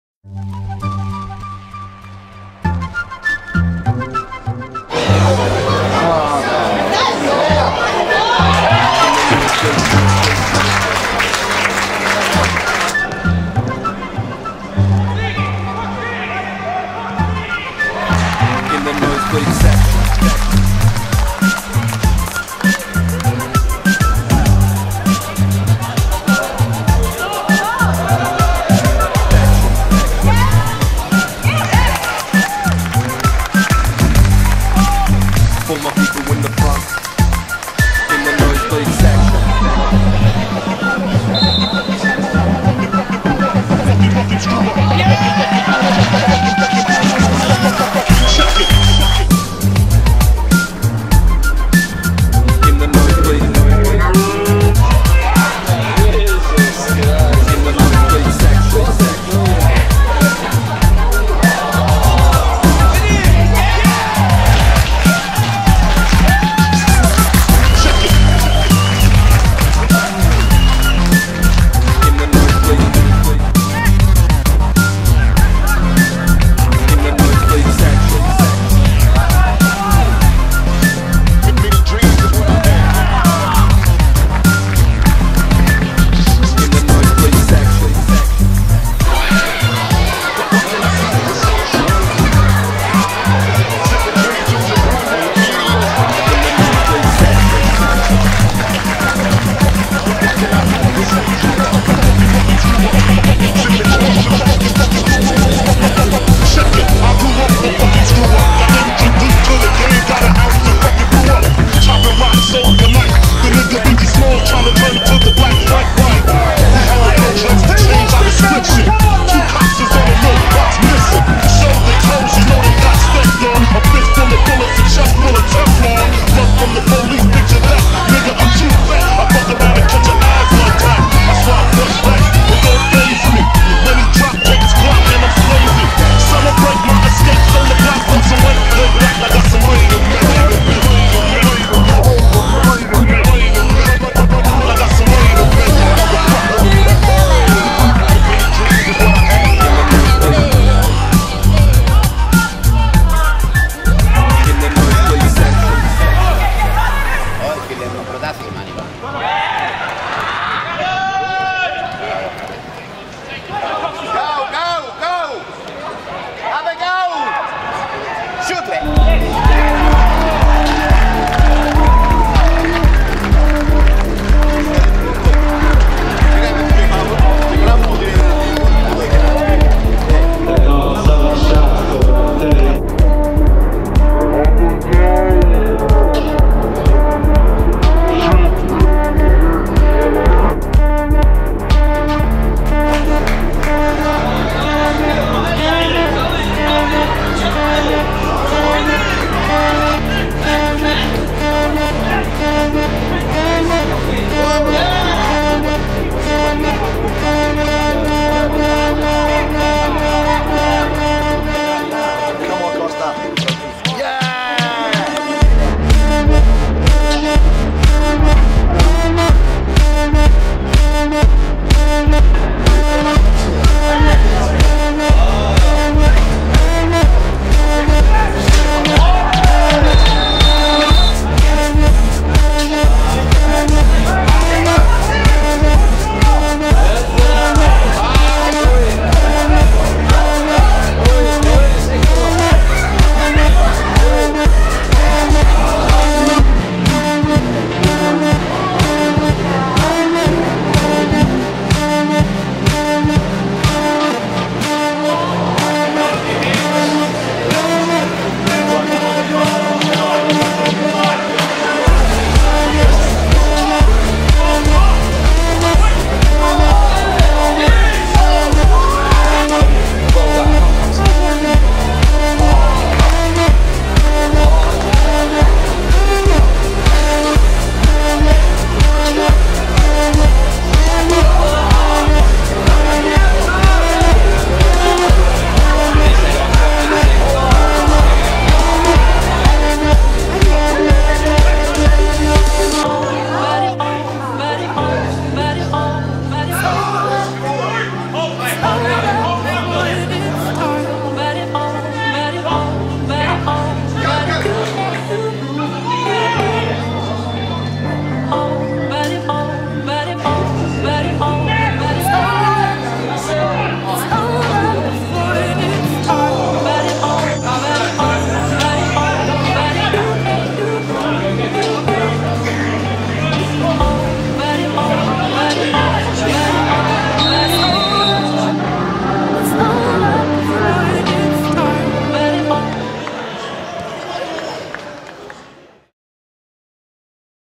Jeremy Cox 2015 season highlights
Jeremy Cox's goals and highlights for Sutherland Sharks u20s and 1st grade 2015